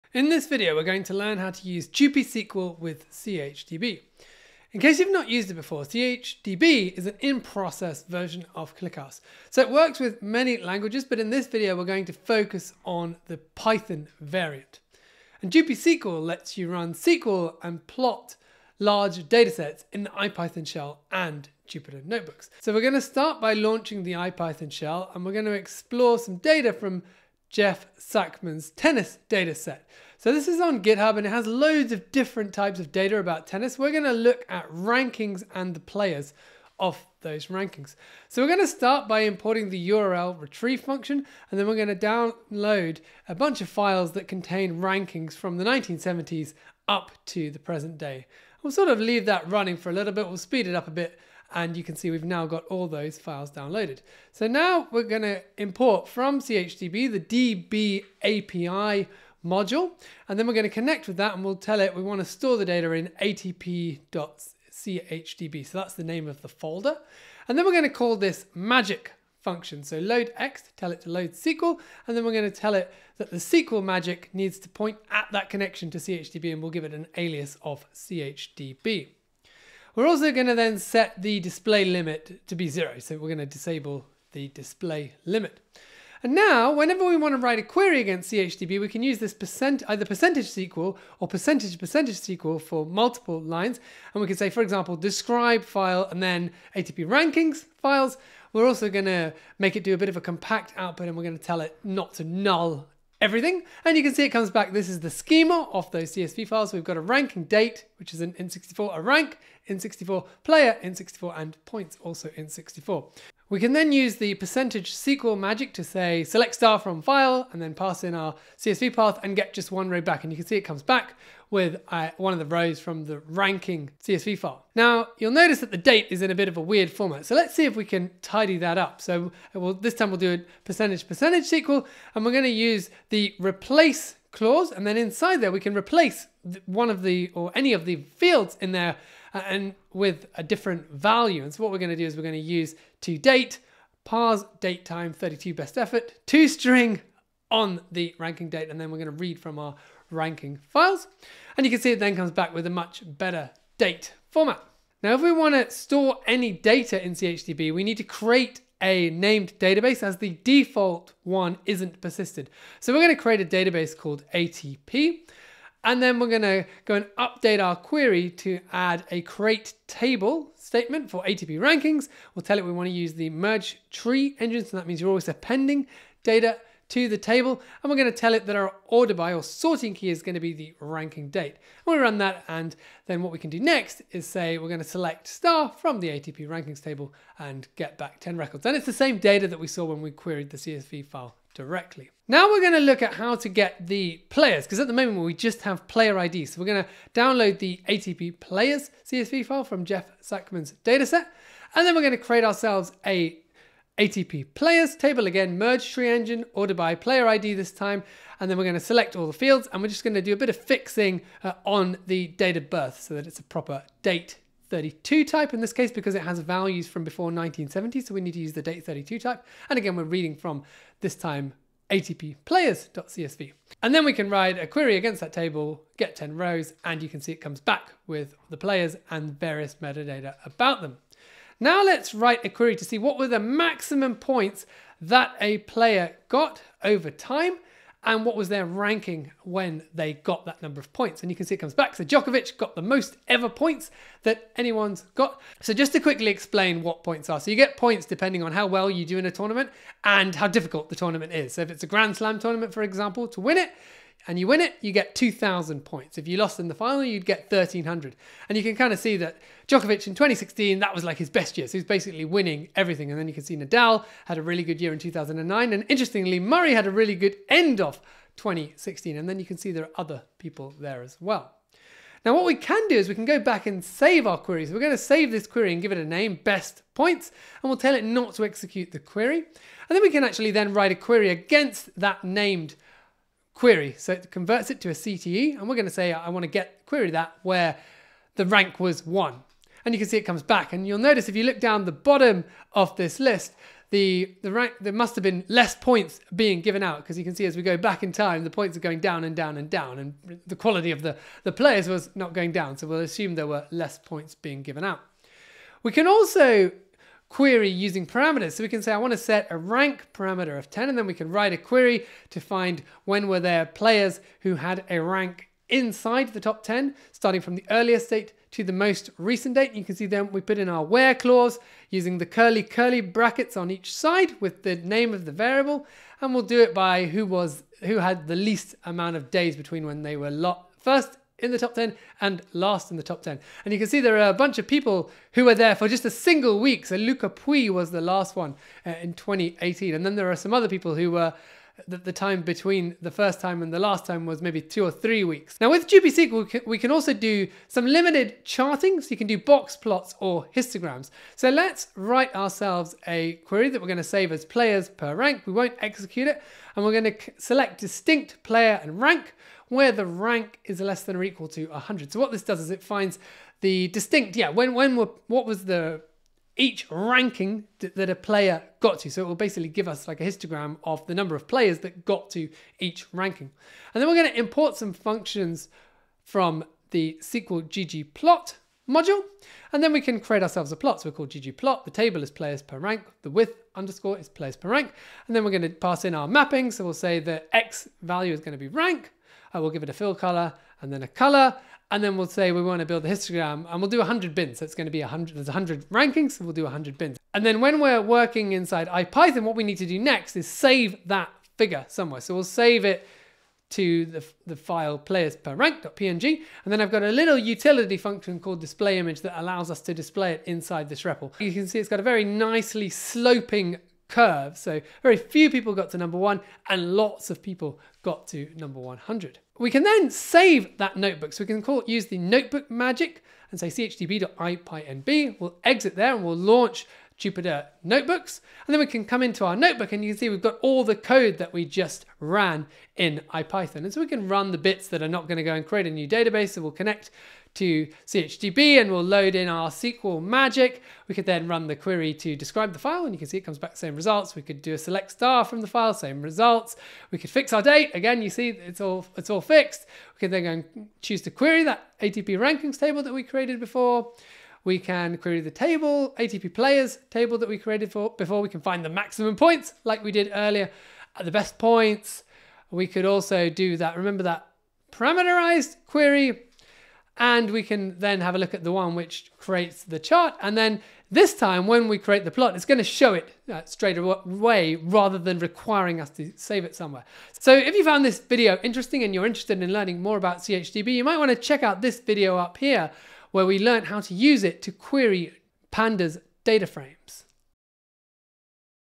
Intro to JupySQL with chDB
This video will teach us how to use JupySQL with chDB, an embedded in-process version of ClickHouse.